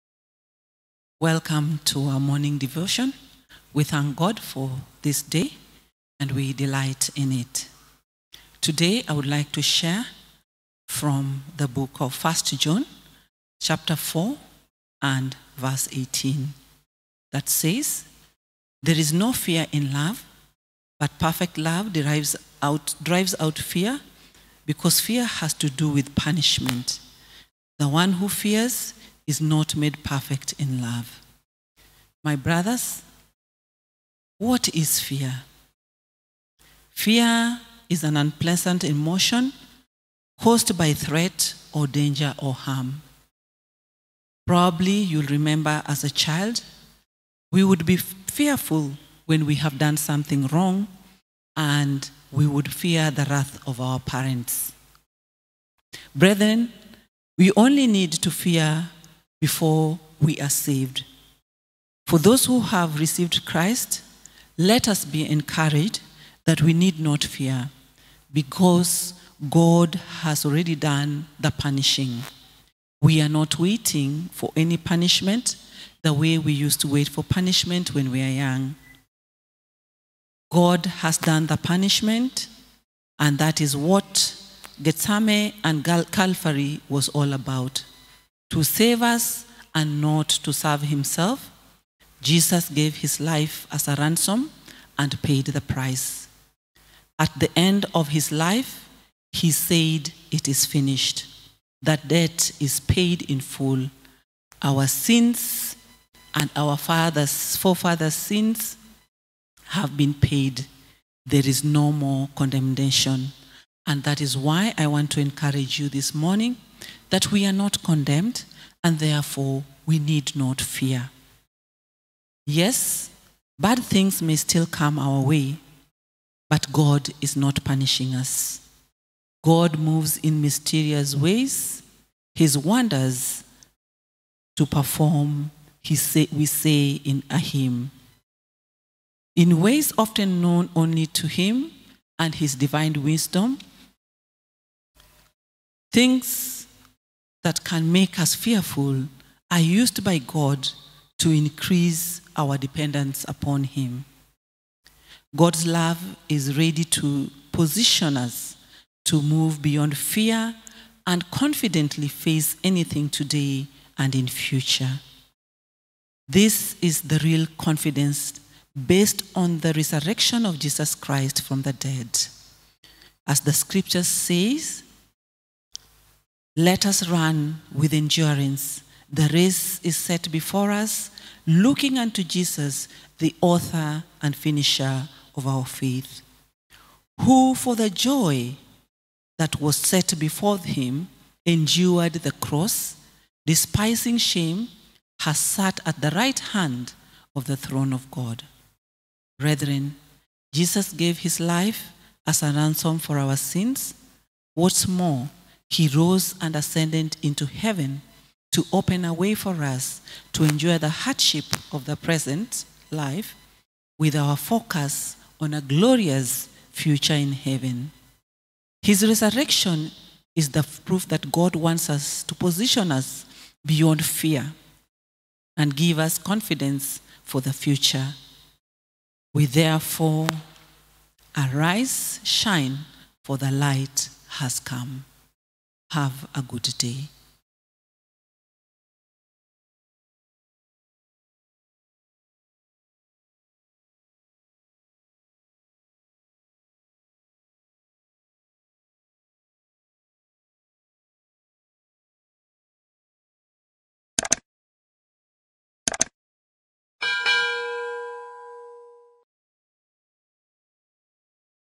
Today's Devotion
Reflection from the book of 1 John 4:18. Lay reader Jane O. shares reflection based on the theme Fear Not, Arise and Shine.